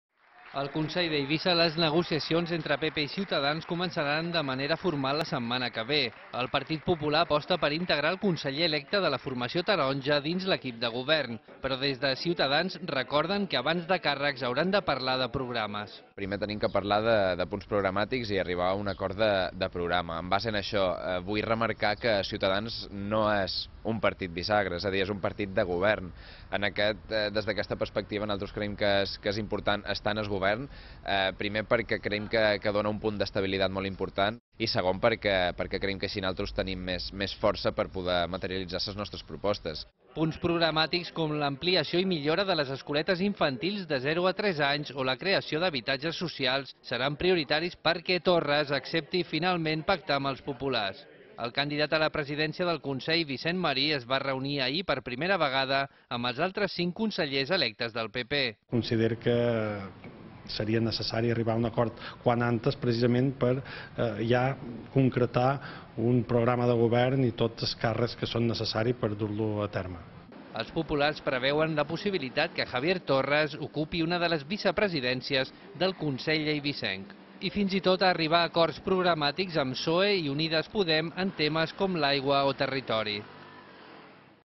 PP i C’s es reuniran els pròxims dies per acordar el govern del Consell d’Eivissa
Punts programàtics com l’ampliació i millora de les escoletes infantils de 0 a 3 anys, o la creació d’habitatges socials, seran prioritaris perquè Javier Torres accepti finalment pactar amb els populars. El candidat a la presidència del Consell d’Eivissa, Vicent Marí, s’ha reunit per primera vegada amb els altres cinc consellers electes del PP.

El PP d’Eivissa té la intenció fins i tot d’arribar a acords programàtics amb PSOE i Unides Podem en temes com l’aigua o territori.